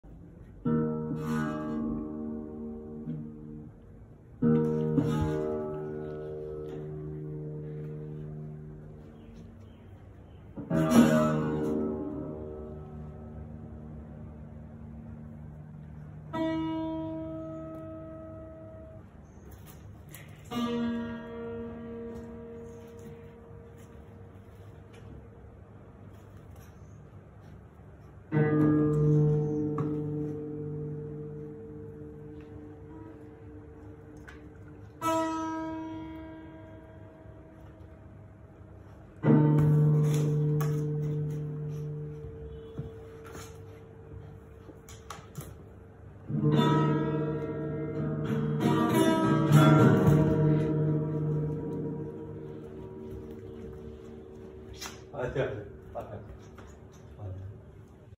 Boxer puppy funny run for life : scared of guitar strings!
01 month old boxer puppy